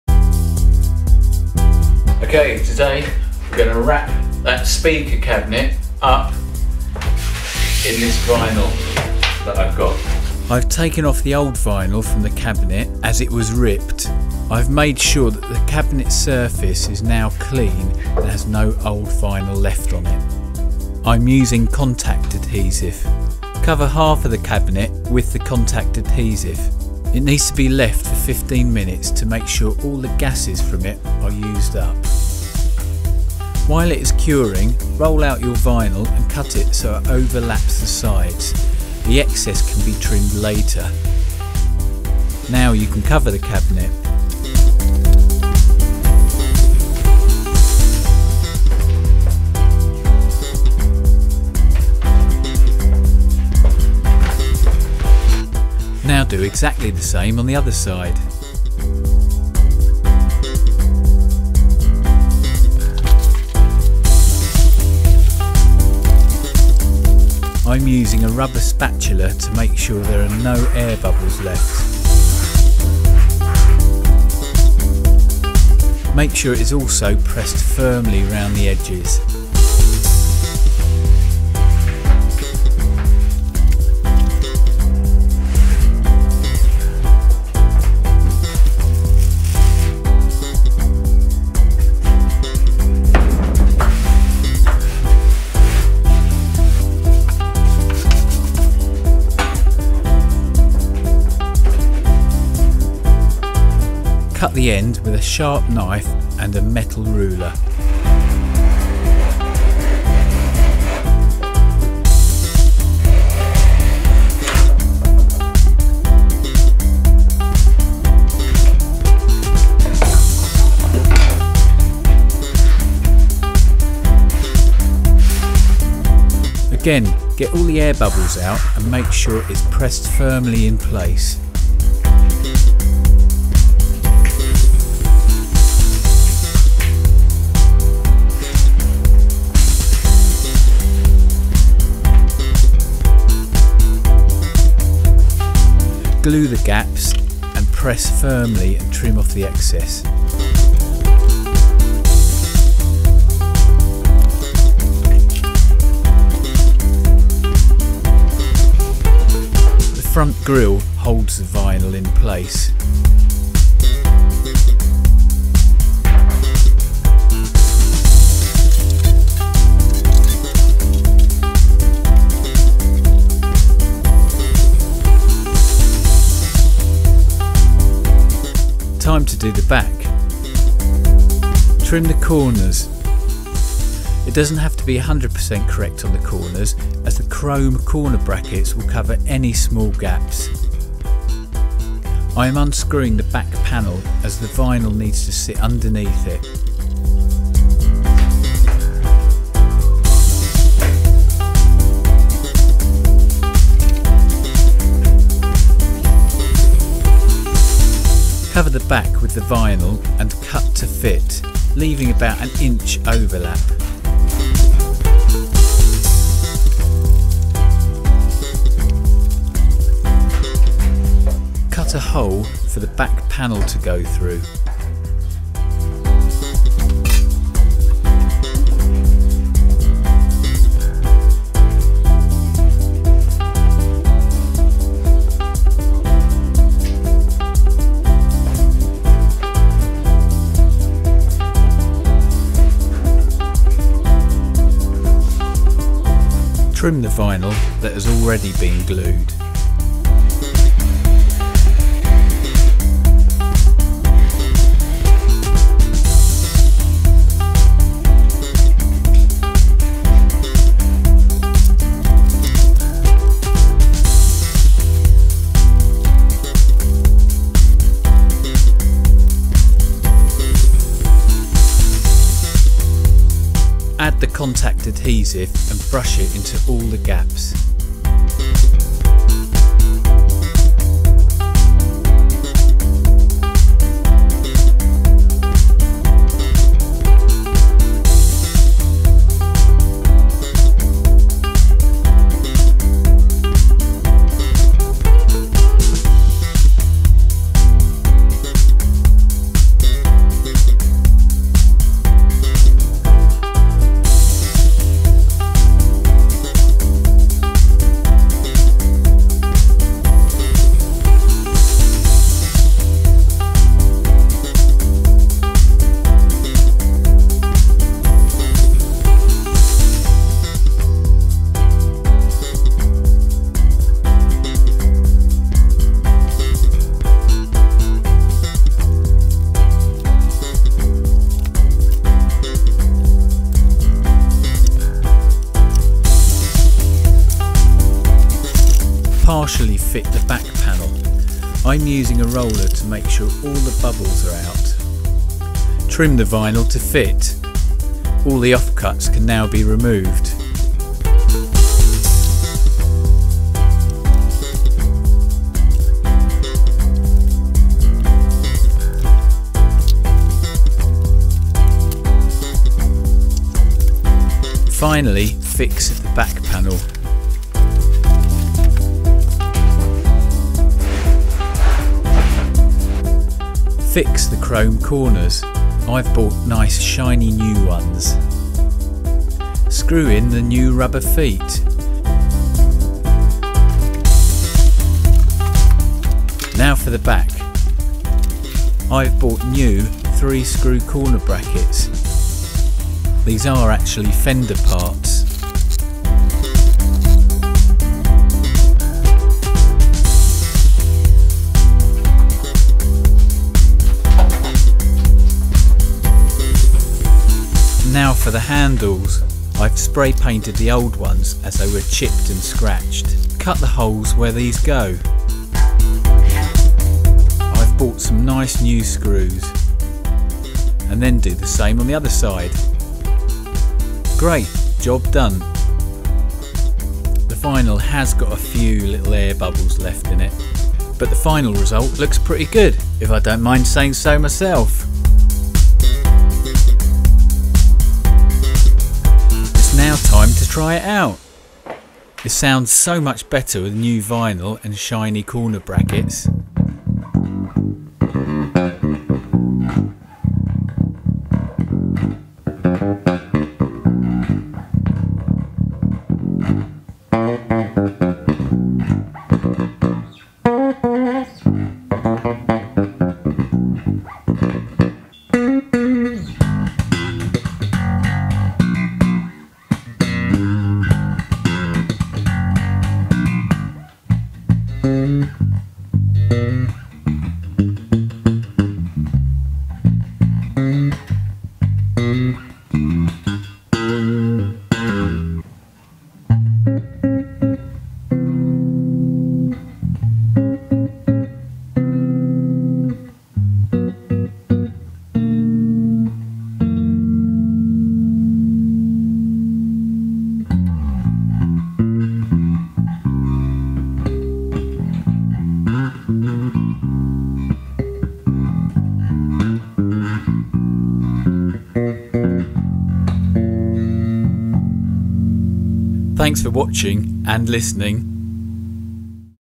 COVERING A SPEAKER CABINET WITH VINYL
This is a video of how to refurbish a speaker cabinet and cover it with vinyl.

I used new corner brackets and refurbished the other parts to make it look like new.